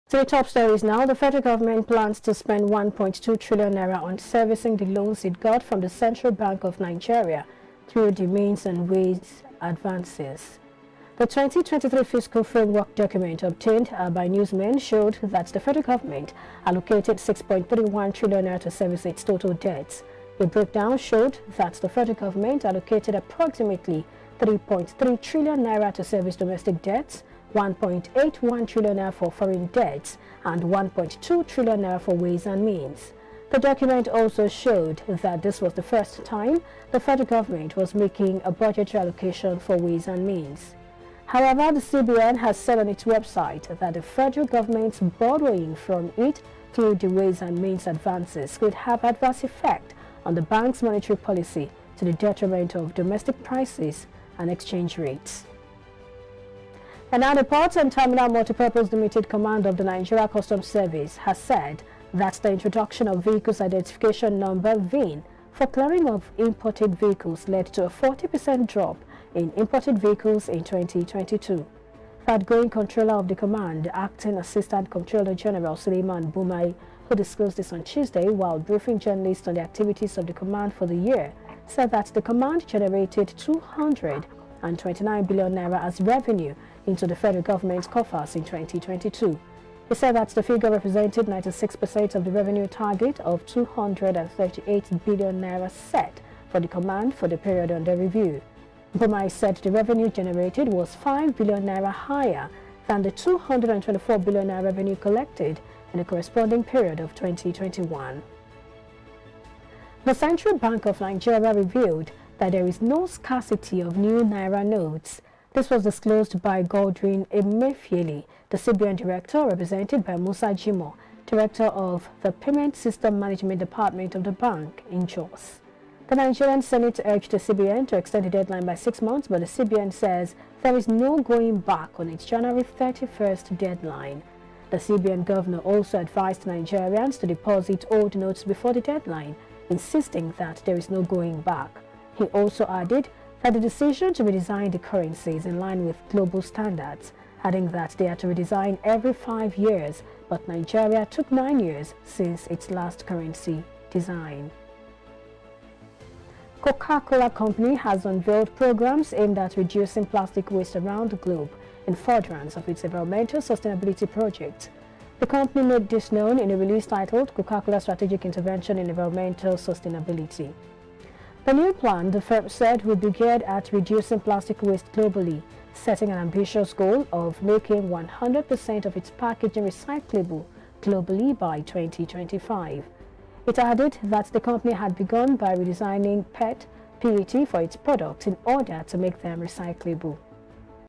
WAYS & MEANS: FG PLANS TO SPEND N1.2TRN ON DEBT SERVICE
The Federal Government plans to spend N1.2tn on servicing the loans it got from the Central Bank of Nigeria through the Ways and Means Advances, findings by The PUNCH have shown.

The 2023 fiscal framework document obtained by our correspondent showed that the Federal Government allocated N6.31tn to service its total debts.

A breakdown showed that the Federal Government allocated approximately N3.3tn to service domestic debts, N1.81tn for foreign debts and N1.2tn for ways and means.

The document also showed that this was the first time the Federal Government was making a budgetary allocation for ways and means.